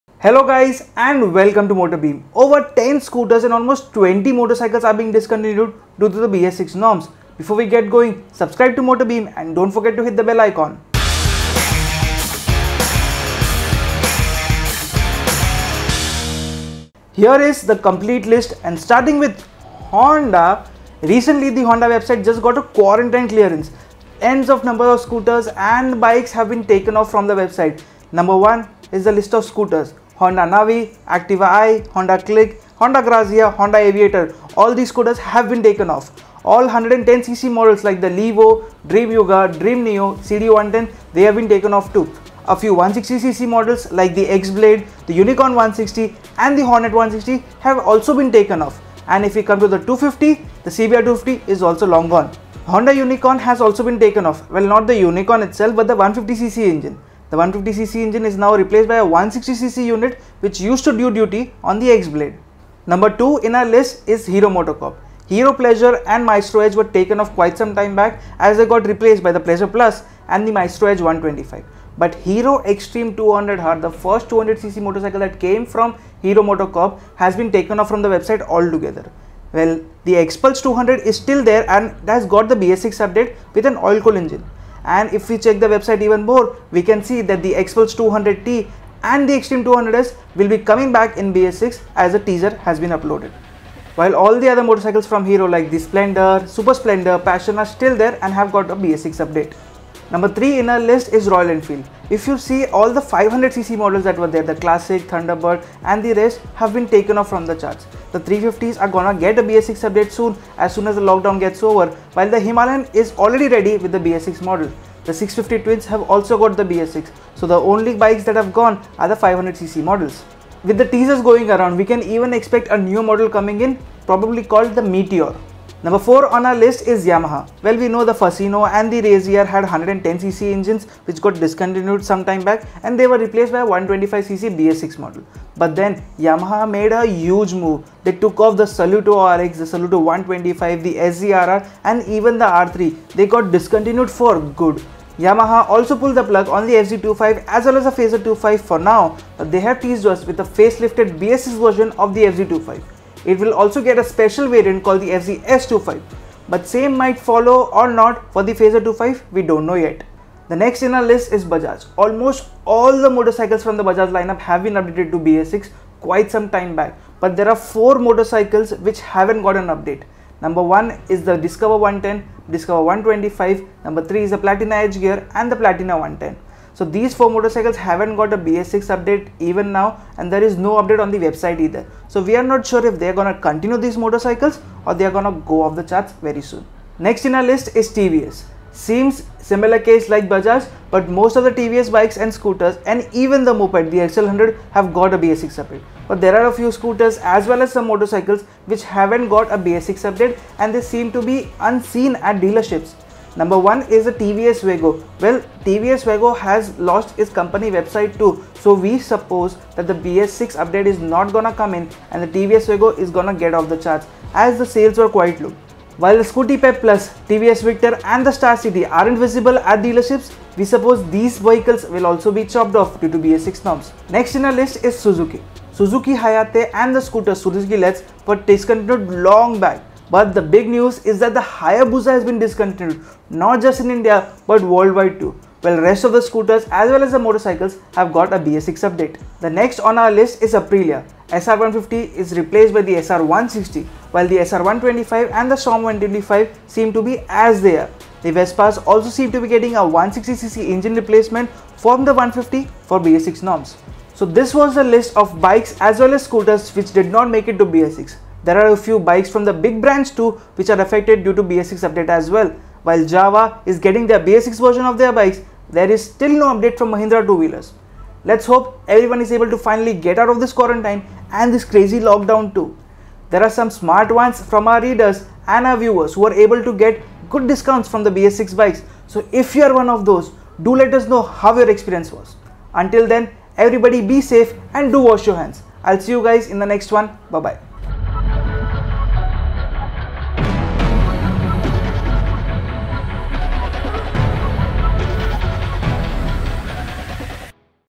Top Bikes & Scooters Discontinued Due To BS6 | MotorBeam
From 1st April 2020, BS6 emission norms have come into effect and this has led to a lot of bikes and scooters to be discontinued from the Indian market. In this video, we tell you all the bikes and scooters from each and every brand being removed from the market.

Become a MotorBeamer and join the MBSquad